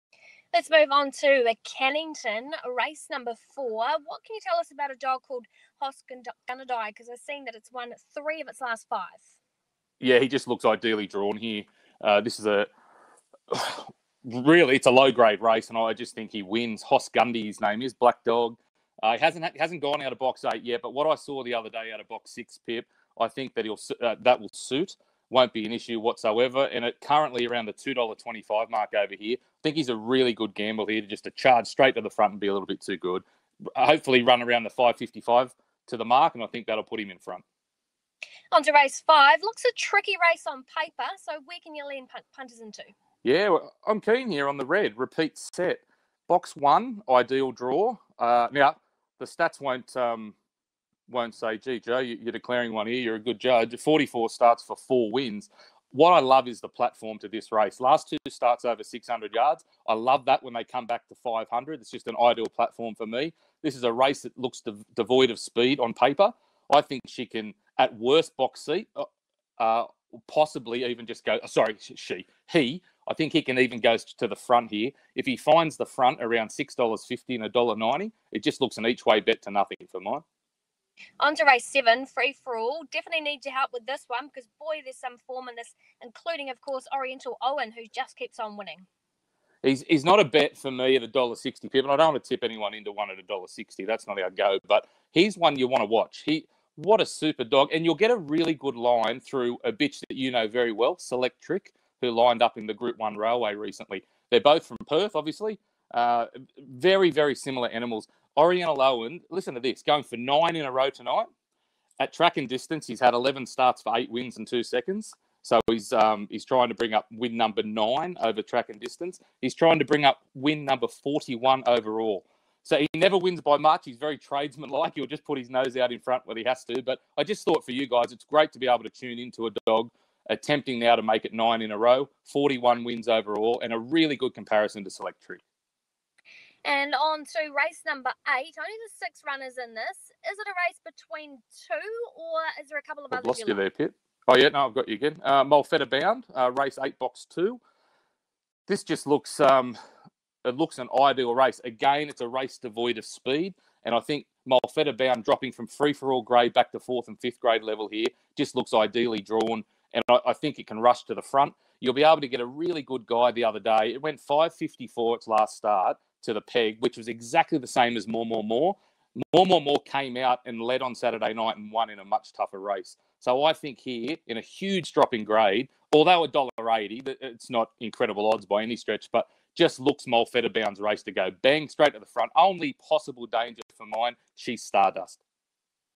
Joe Lannutti: Cannington 30 March
Pip catches up with Joe Lannutti to talk Cannington Dogs.